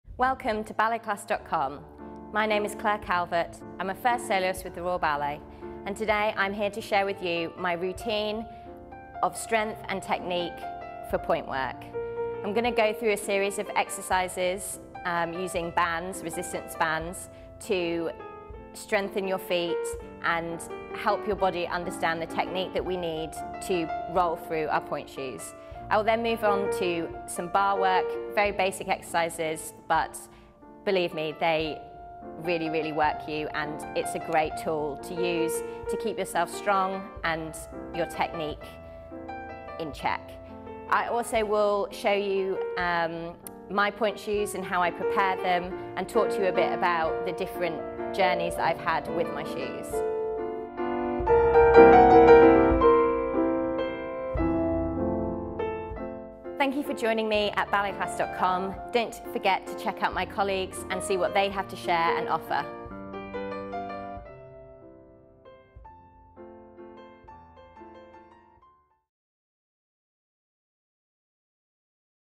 Claire Calvert - Pointe Masterclass teaser
Claire’s first class for balletclass.com is a basic level Pointe Masterclass covering everything Pointe! Claire has created this class herself and begins the class by sharing her own routine of strength exercises for improving pointe technique. As well as talking through her own personal pointe shoe journey, Claire reveals how she personally prepares her pointe shoes before wearing them and dancing in them. On top of all of those exciting things, Claire finishes the class by putting all of her tips into practice and demonstrates a specially designed Basic Pointe Barre with exercises created to help your body learn and understand the technique required to roll through your pointe shoes!

The masterclass which this teaser is advertising is available now to subscribers